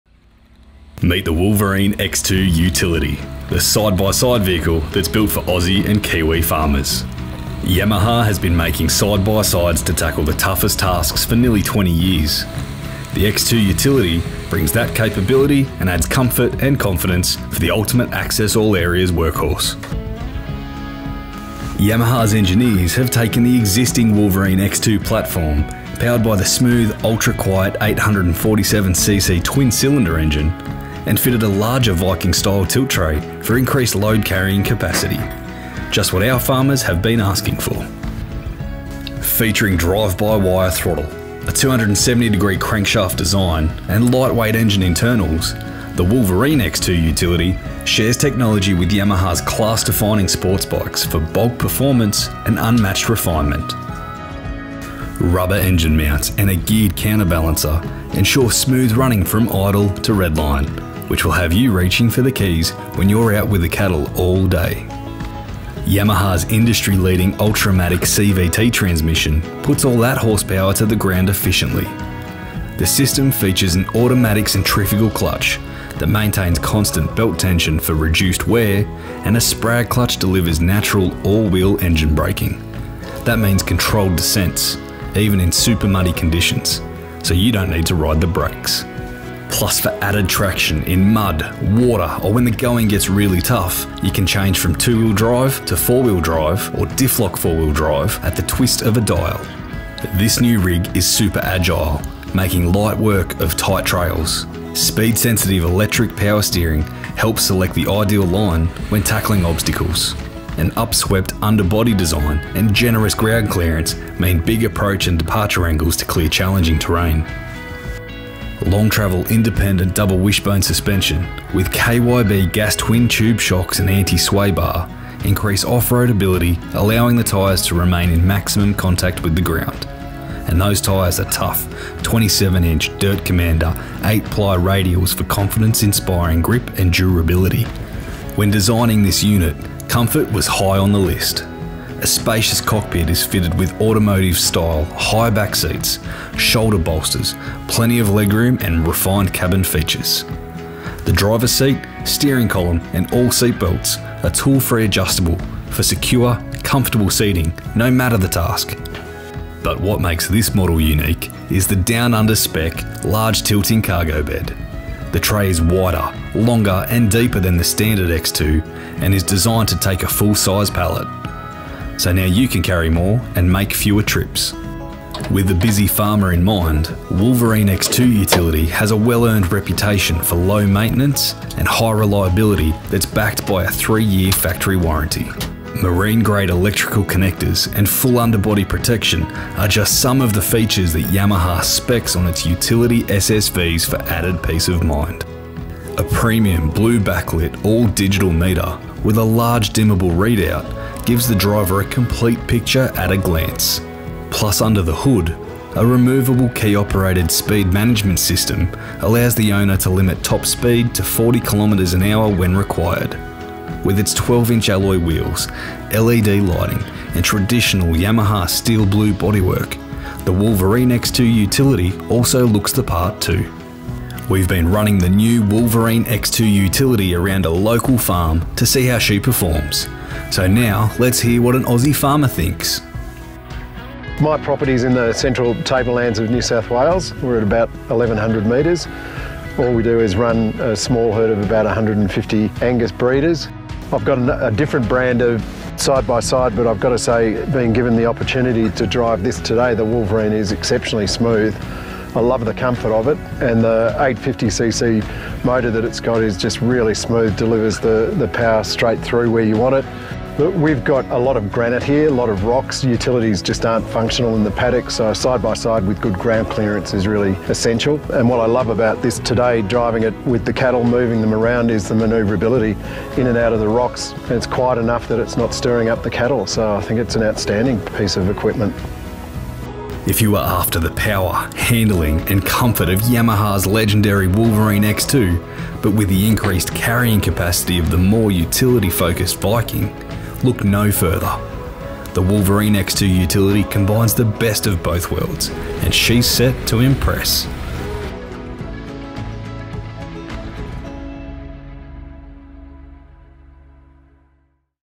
Wolverine X2 Utility
Designed for the Aussie farmer and featuring Yamaha's legendary Real World Tough durability and reliability, the Yamaha Wolverine X2 Utility prioritises work over recreation by featuring a larger cargo bed than standard - the same hydraulic-assist tilting device fitted to the hard working Viking three seater utility SSV.